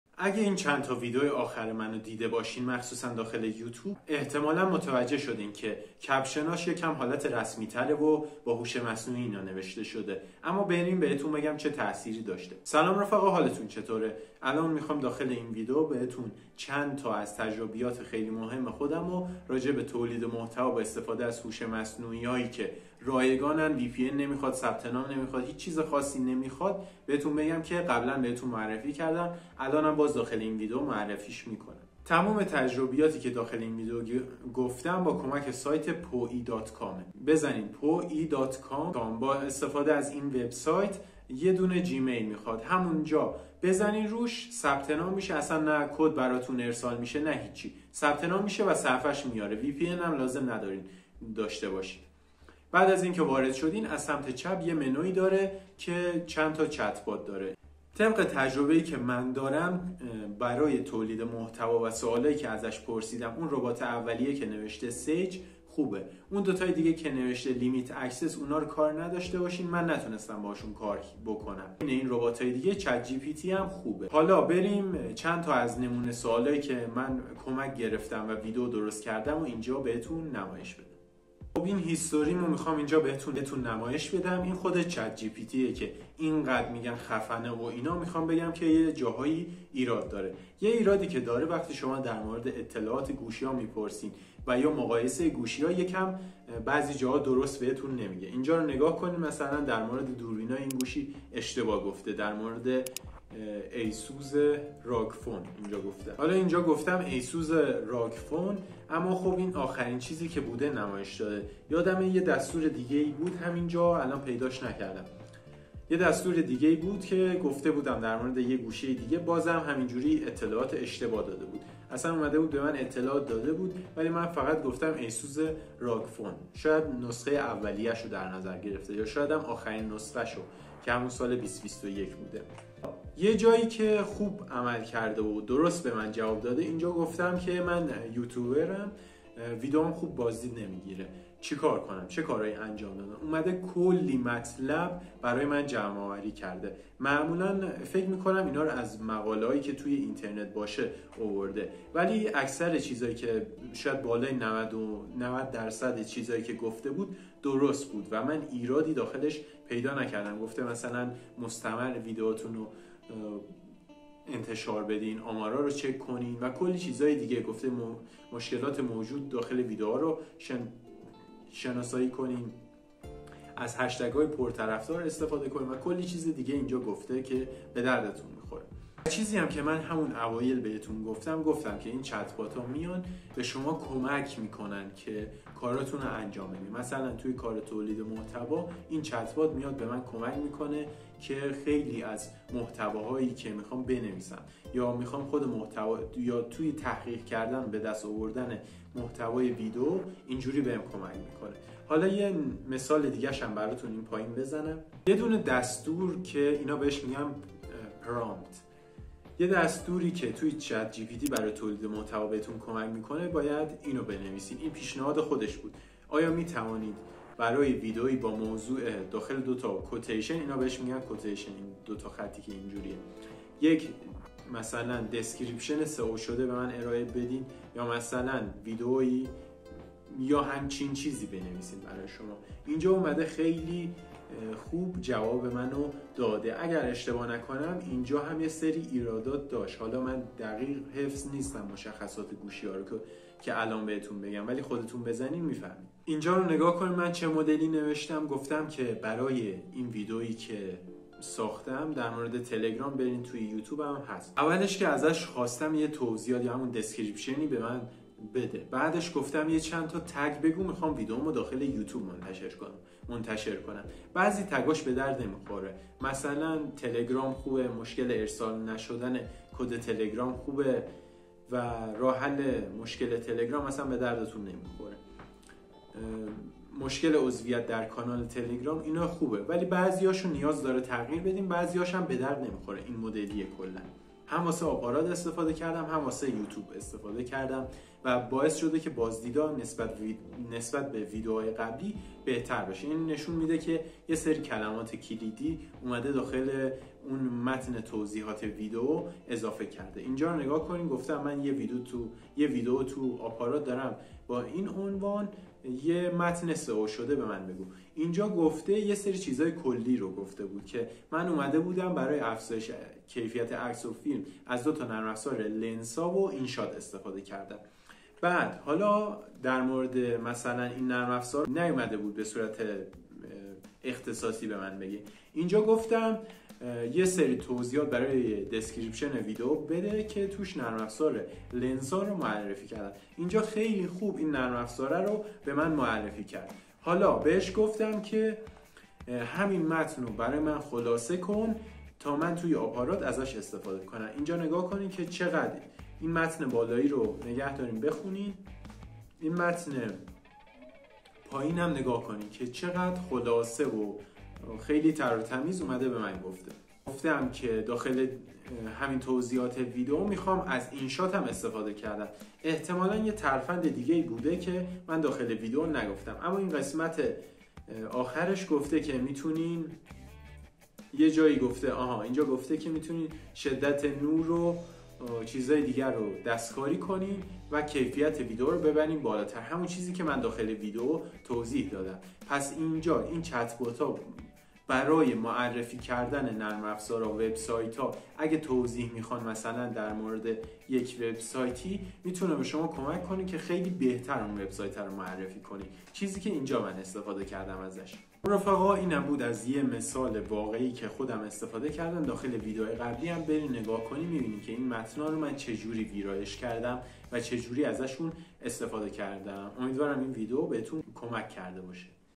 Chat GPT: تجربیات من از تولید محتوا با هوش مصنوعی
قبلا هم به شما این وبسیات رو معرفی کردم که با کمک اون میتوانید به صورت رایگان از چت جی پی تی استفاده کنید و کلی چت بات دیگه هم داره برای تولید محتوا عالی هستند ومیتونی کلی محتوا با کیفیت و خلاقانه با کمک این وبسایت بسازید
نکته : در حال حاظر این وبسایت اشتراکی شده و رایگان نیست !
00:00 - مقدمه
01:21 - تجربیات من در تولید محتوا با هوش مصنوعی
06:38 - جمع بندی

شما همم تست کنید بگید کدوم چت بات این وبسایت بیشتر به شما کمک کرده
و یا اگه جایی مشکل داشت بگید .

لینک شبکه های اجتماعی من:
اینستاگرام من: